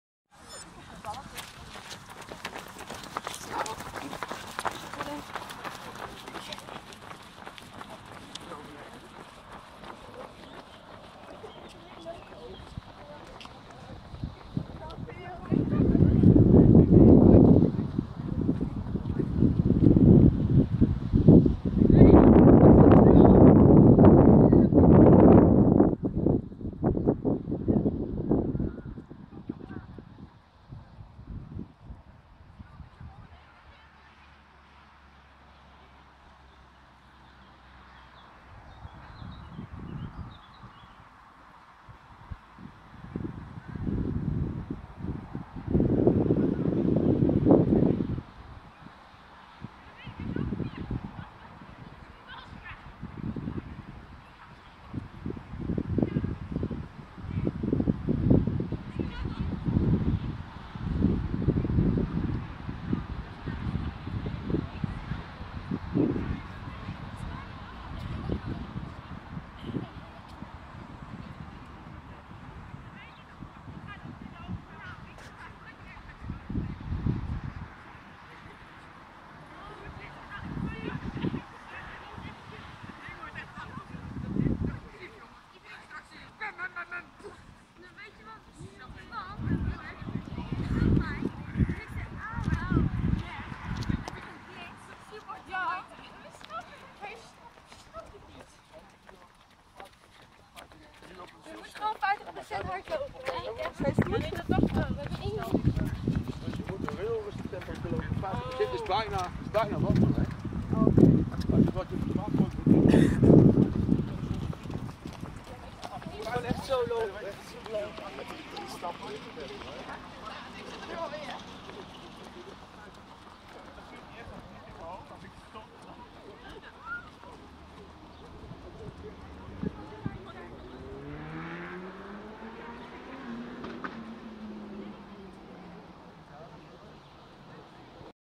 Zoladz test
Zoladz test tijdens droogtraining op parkeerplaats de Oude Vos.